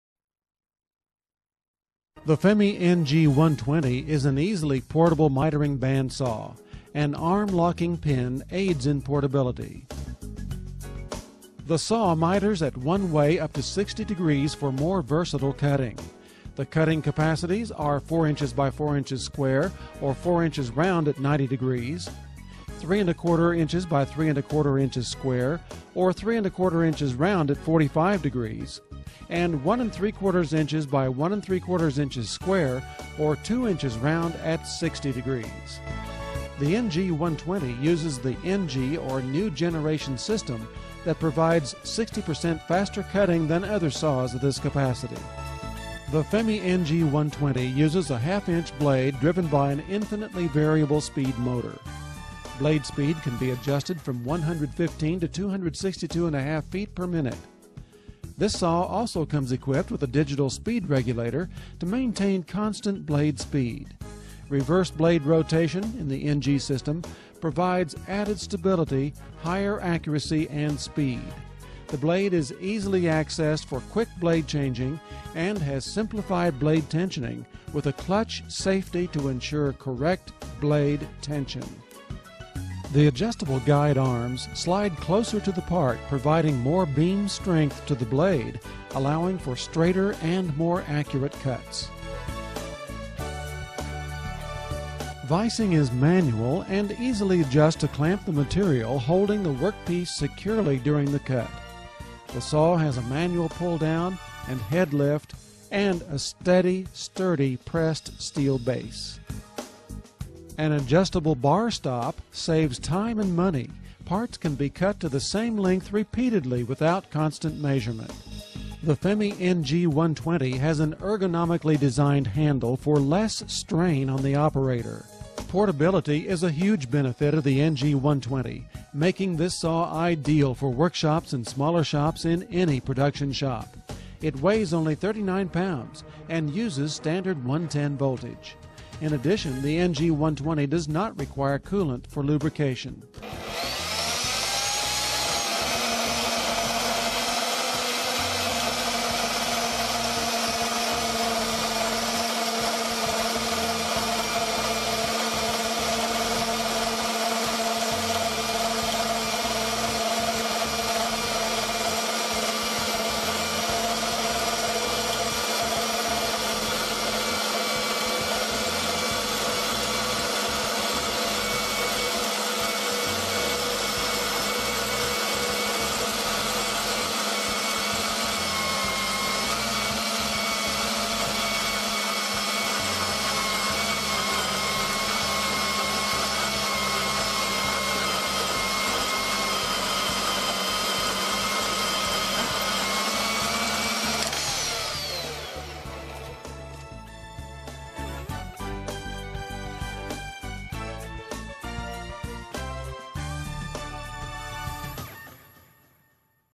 TOOL MART QATAR - FEMI BAND SAW NG120
Great versatility, technology and performances, NG System up to 60% faster

-max cutting capacity Ø 120 mm - 120x102 mm
-weight Kg 18
-bandsaw blade mm. 1440 x13 x0,65
-NG System: reduction of cutting time up to 60% !!